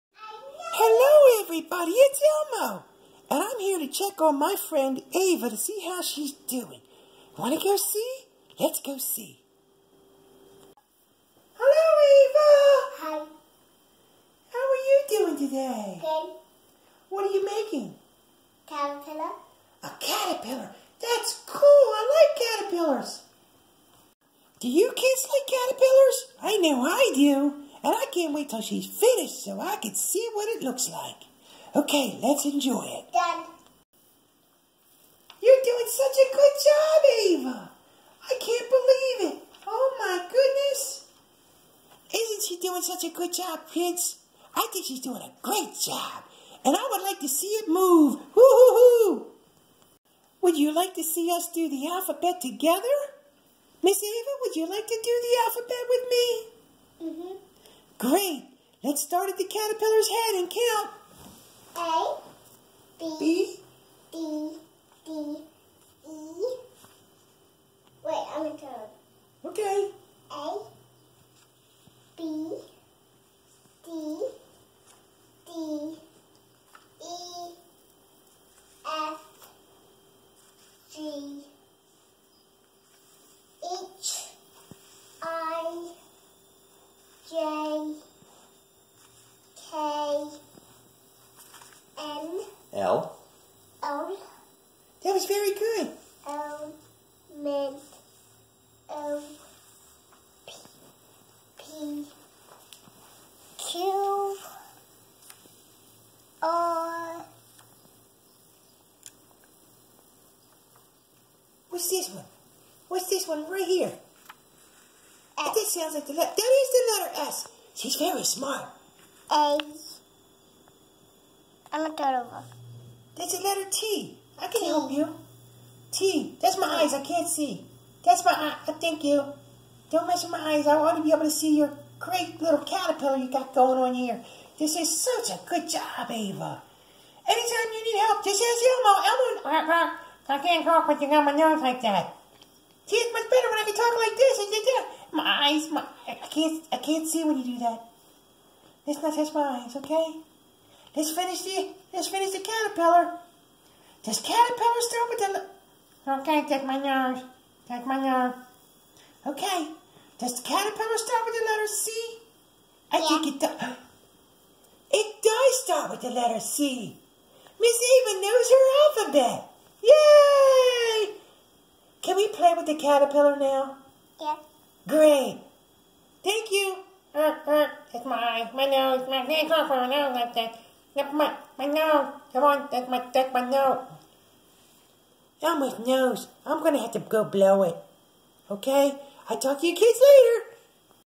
Elmo checks in on his friend, Ava to see how she's learning her alphabet.
Learning with Elmo. Voiceover by@SpiderLutz John Spider Batman Lutz. The caterpillar lesson on the alphabet fun! Elmo's poor nose! 😂